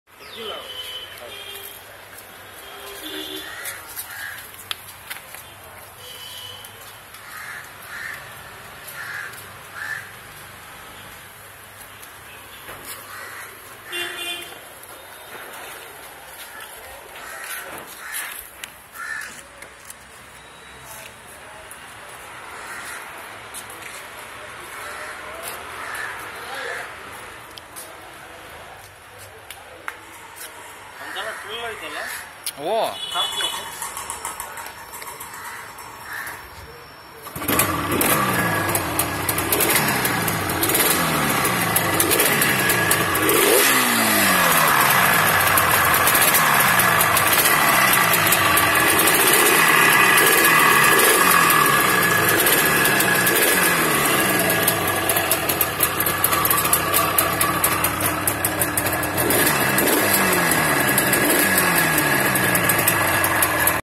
Yamaha RD350 1983, the legend
Will take this bike out for a spin. Should be epic.